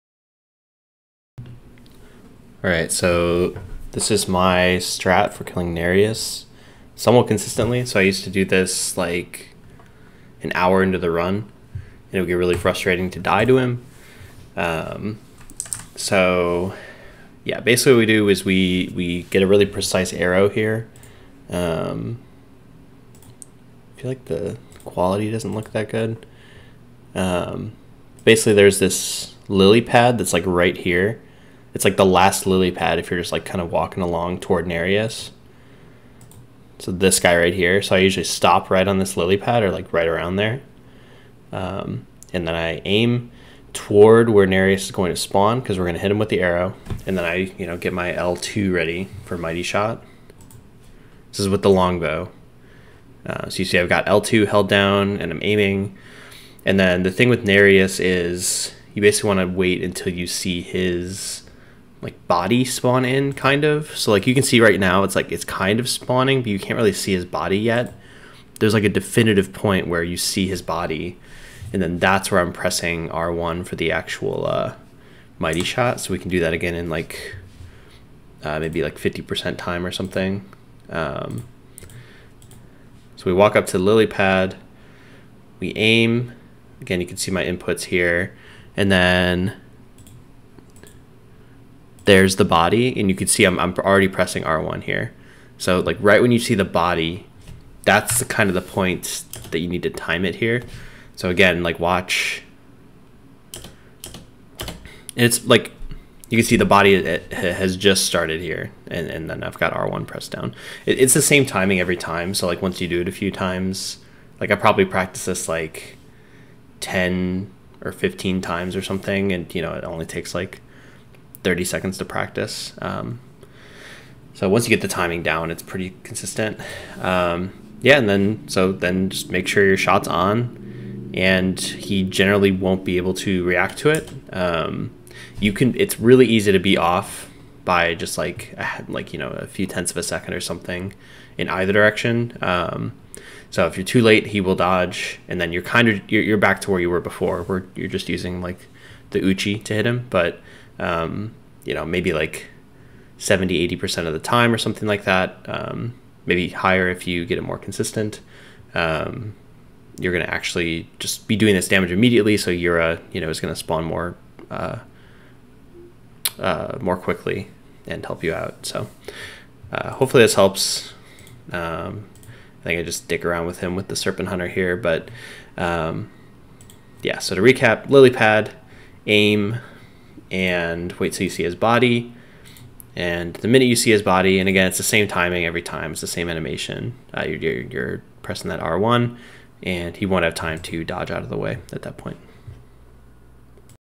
How to hit Nerijus with a bow
How to hit Nerijus with a bow somewhat consistently. If this fails, you're back to normal strats, but it can get you an advantage by getting Yura to spawn faster